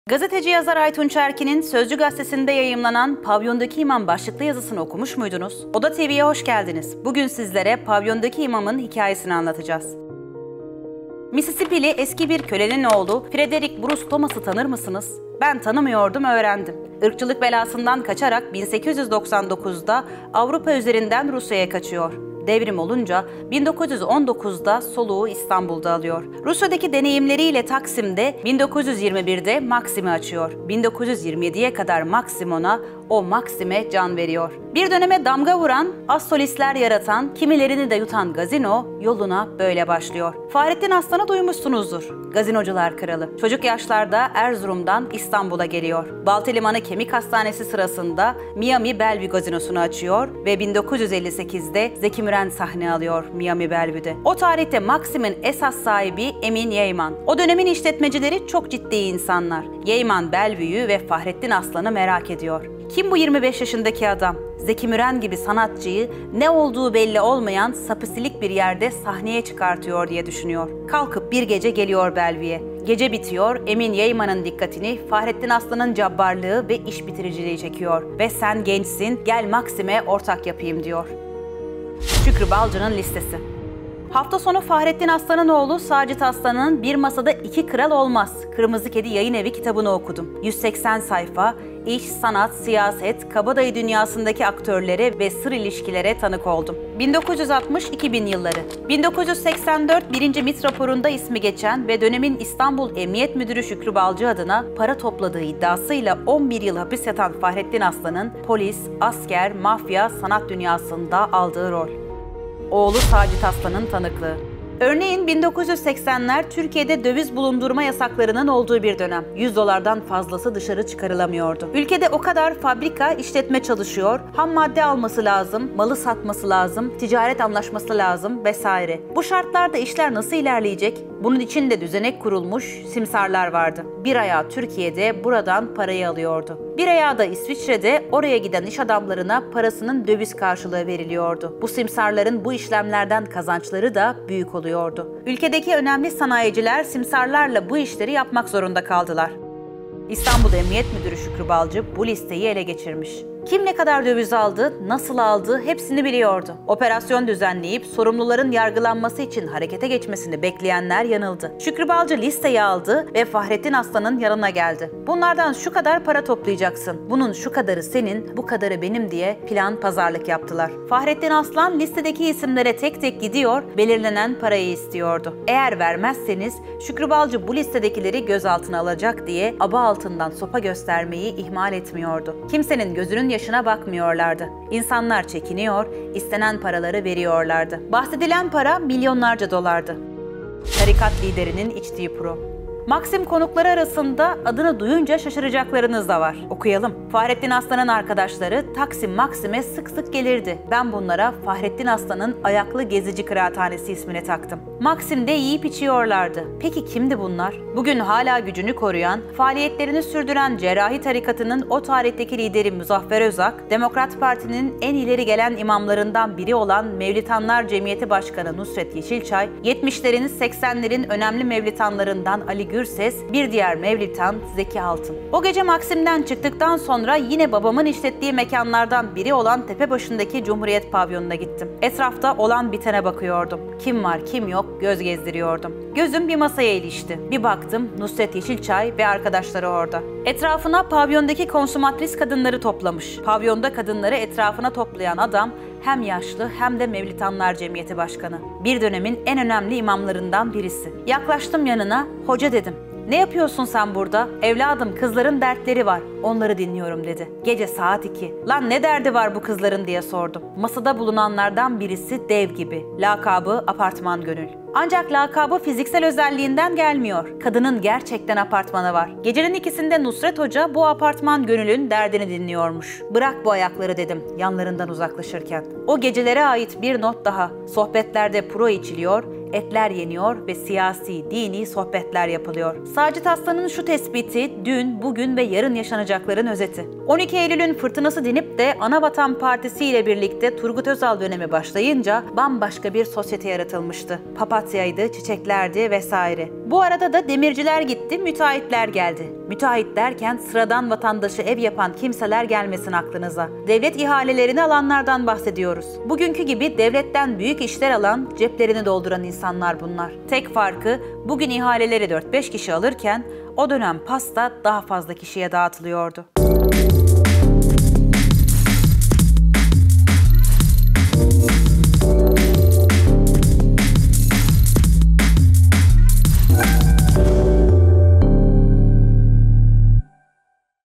Pavyondaki İmam
Gazeteci yazar Aytunç Erkin’in Sözcü gazetesinde yayımlanan “Pavyondaki imam” başlıklı yazısını okumuş muydunuz? Odatv’ye hoş geldiniz…
Bugün sizlere Pavyon’daki İmam’ın hikayesini anlatacağız.
Seslendirme: Pınar Saraçoğlu
Kurgu: Sibel Yıldız